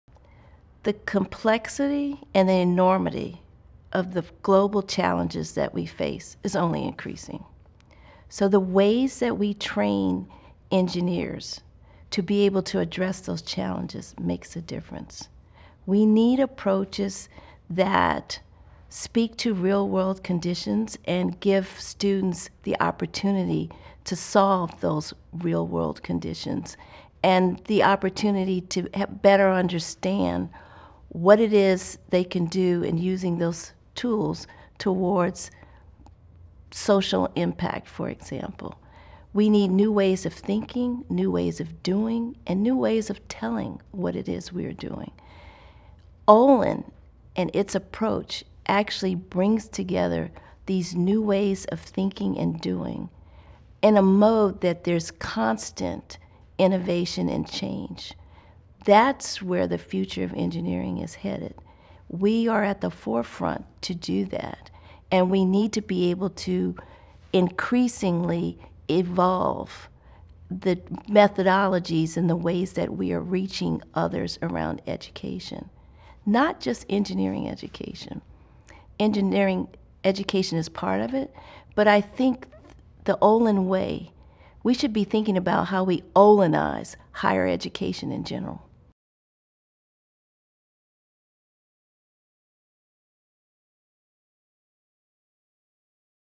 Training Engineers for the Future: Olin's President-elect Shares Approach
On behalf of the Olin College Presidential Search Committee and the Board of
Trustees, we're delighted to announce the selection of Dr. Gilda Barabino as the second
president of Olin College, effective July 1, 2020. Dr. Barabino’s appointment comes after a
comprehensive search that drew interest from around the world.

"The complexity and enormity of the global challenges we face is only increasing. So the ways we train engineers to address those challenges makes a difference - we need approaches that speak to those real-world conditions and that give students the opportunity to solve them." Meet Olin College's new President-elect, Dr. Gilda Barabino!